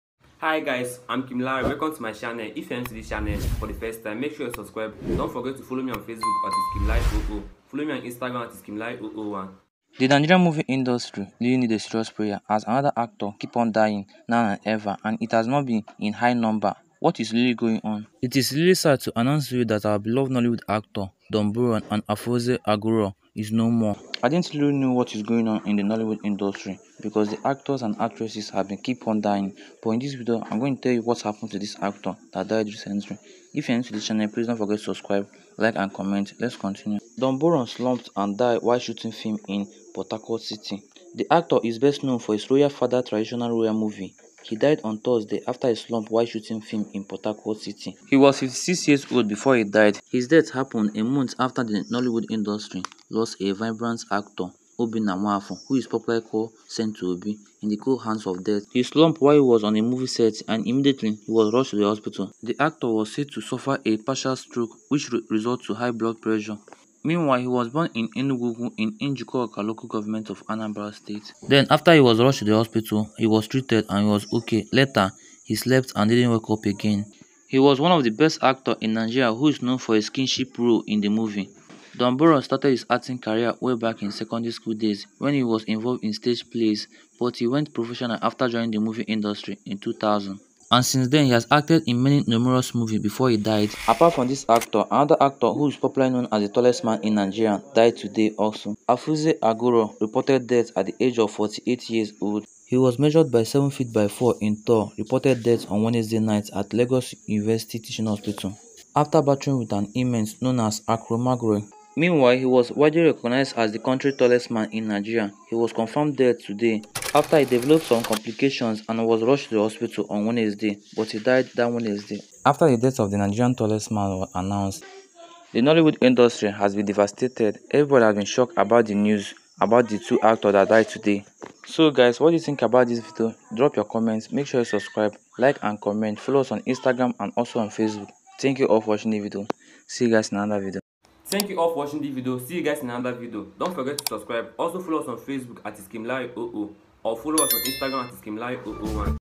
Secrets Behind Don Brymo & Afeez Agoro Death Finally Revealed
The Nigerian Movie Industry Really Need a serious prayer as another actors keep dying every moment, it's really sad to announce to you that Nollywood Actor Don Brymo & Afeez Passed Away 😭

Join this fanbase to get access to custom emojis, royalty badges & priority replies to comments.